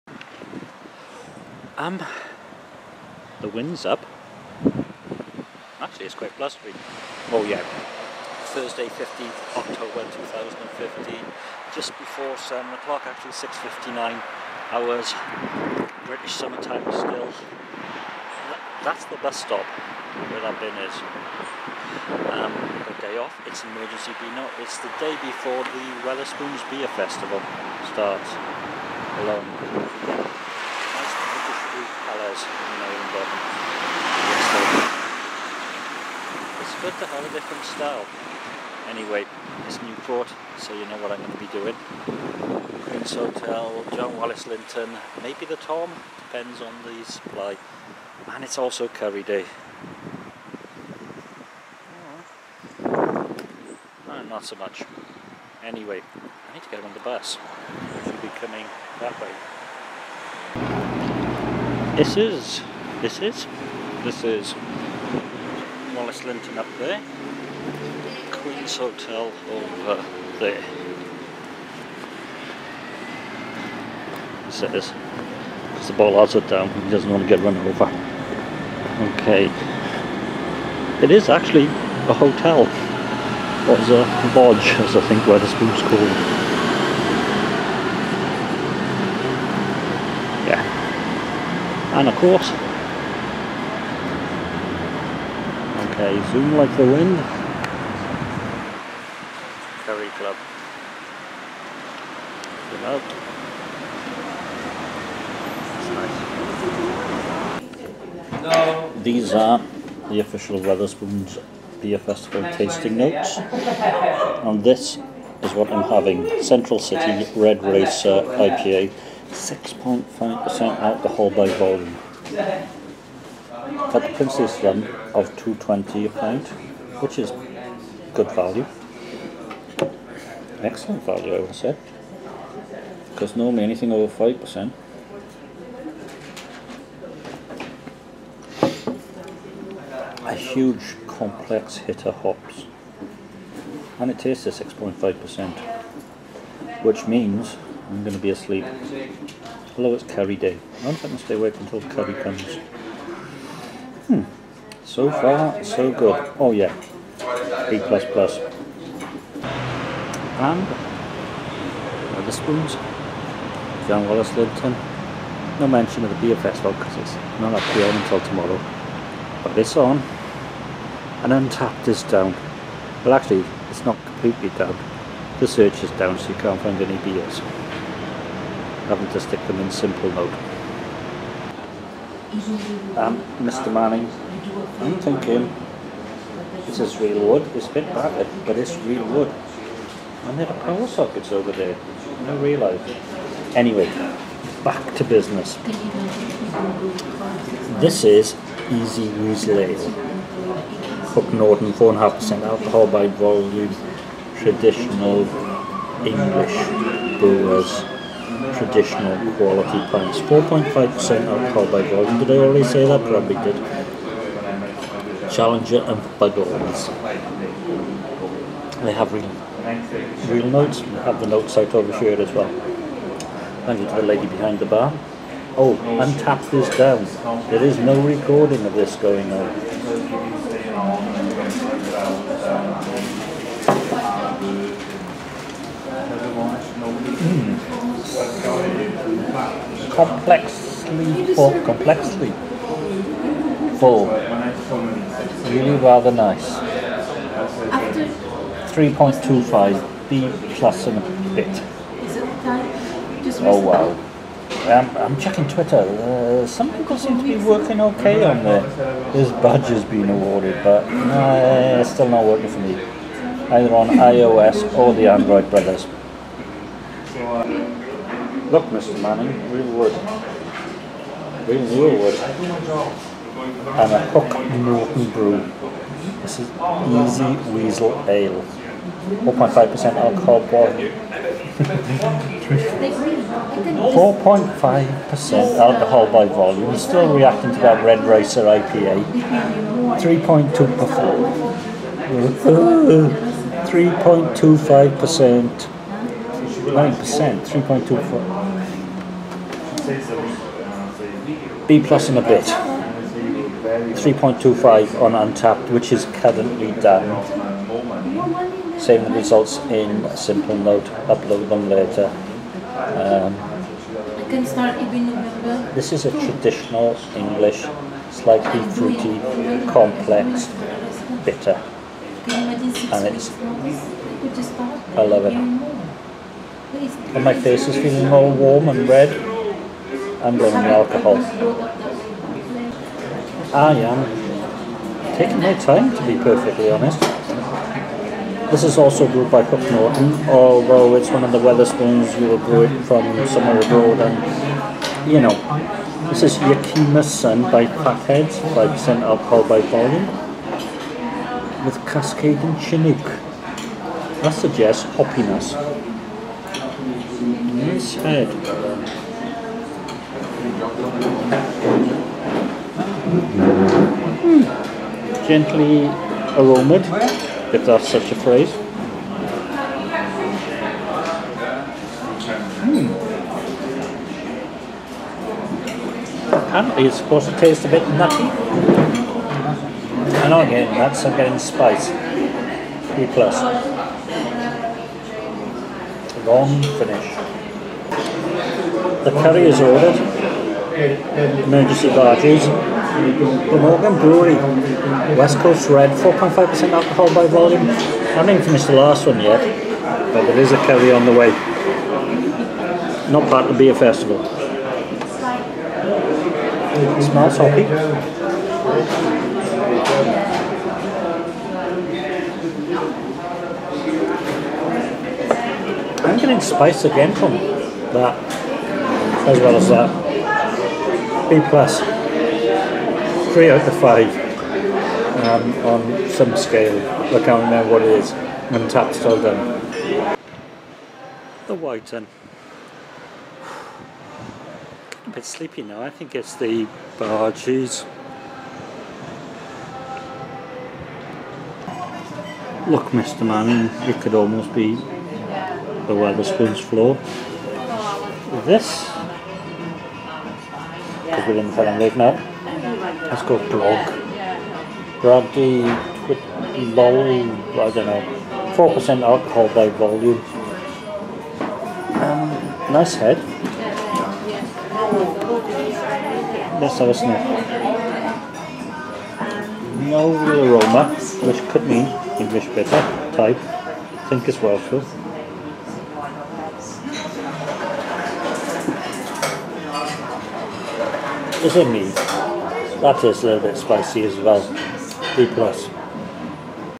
Can You Drink Without The Internet
Got in early the day before the Wetherspoons Autumn Beer Festival started. Newport, standard plant but minus the Lamb and the Harbour Fish bar. Added was a Curry Thursday, Spoons style. Untappd was down until lunch time.